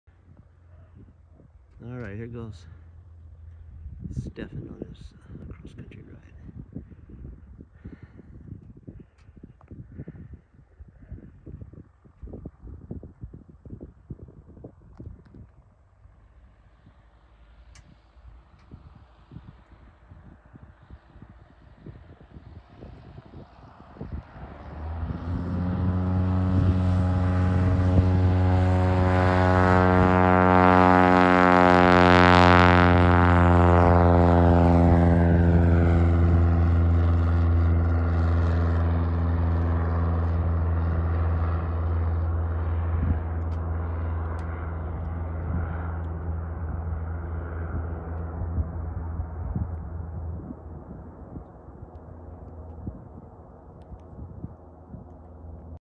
Departing Soldotna for First Solo Cross Country.
Departing Soldotna for Kenai to get three towered airport landings, then down to Homer, Seward, then back to Homer for the night.